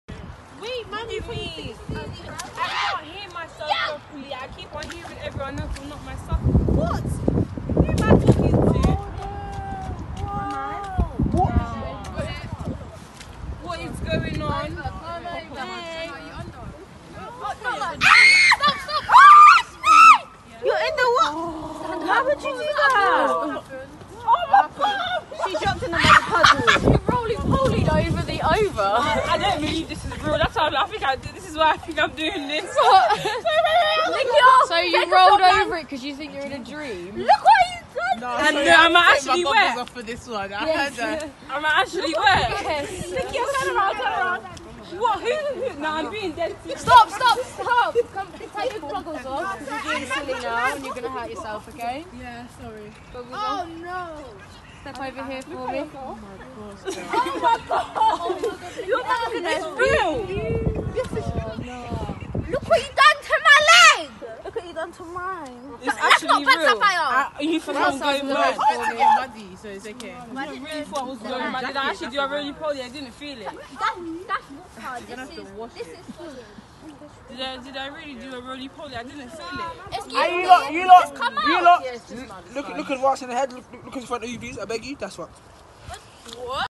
Leadership Residential Programme with @CEFLyncx - "CHALLENGE COURSE" Pt 11 @Woodrow High House
Youth Forum at Blenheim Gardens and Roupell Park RMO - The SW2 Partnership. Youth Leadership Residential Programme with @CEFLyncx - "CHALLENGE COURSE" @Woodrow High House.